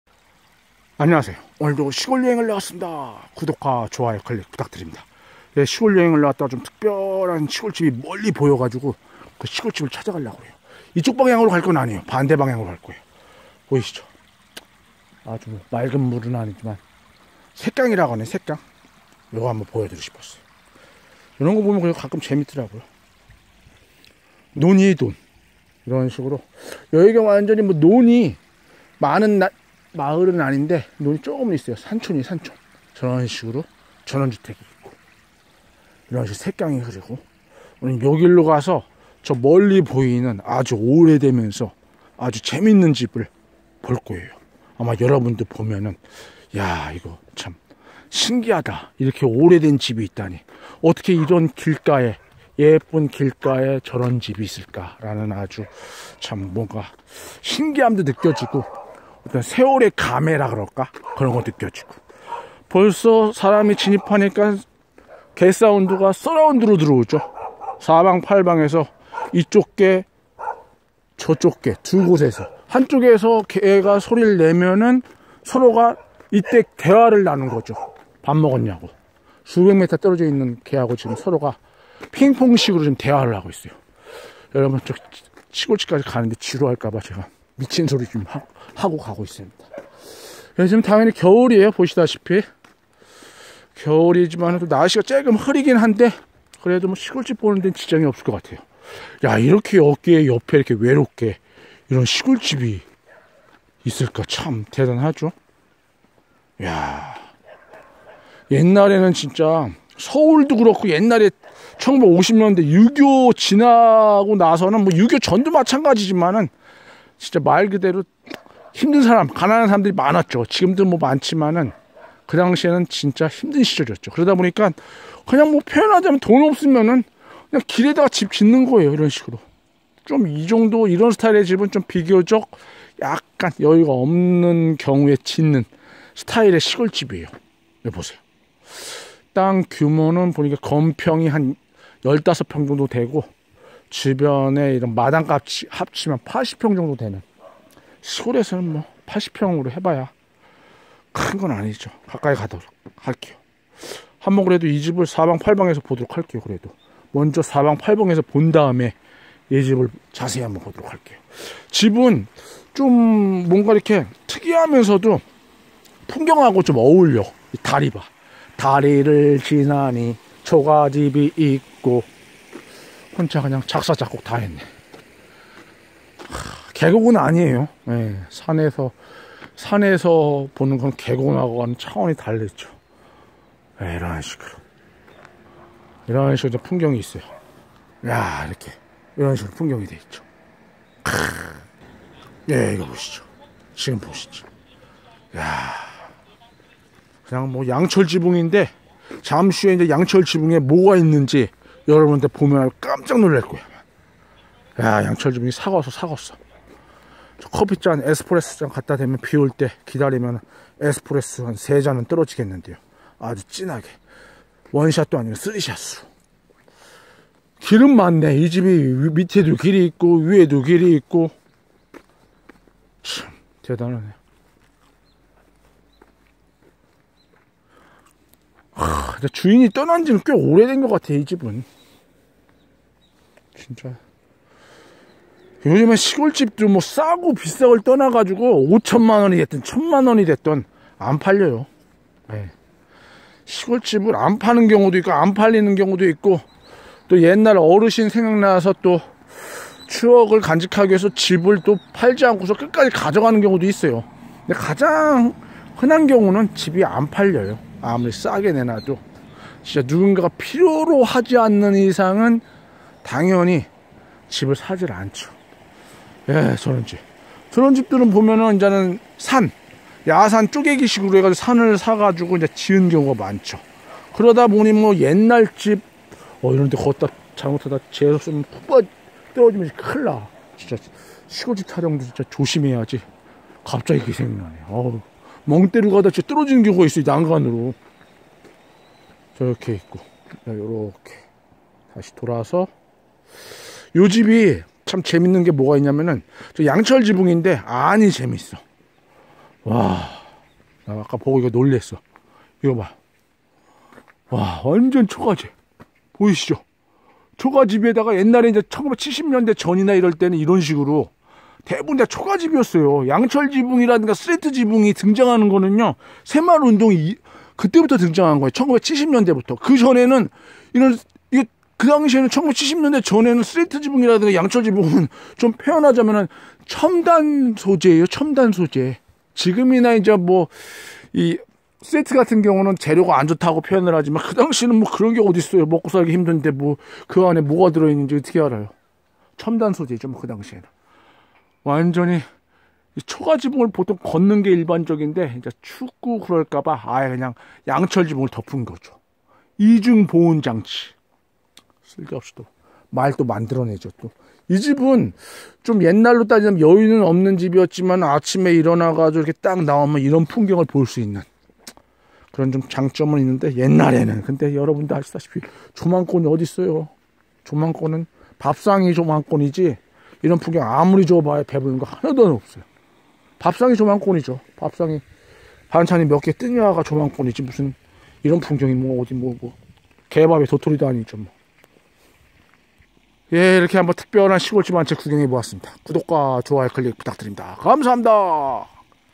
예쁜 길옆에 특이한 시골집이 20년동안 방치! 5백만원에도 팔리지 않아! 풍경 좋은데 아까워! 멋진옛날집 옛날골동품 멋진여행
옛날시골집 멋진옛날집 멋진여행 멋진풍경 옛날풍경 옛날골동품 시골들판 시골부자 시골산책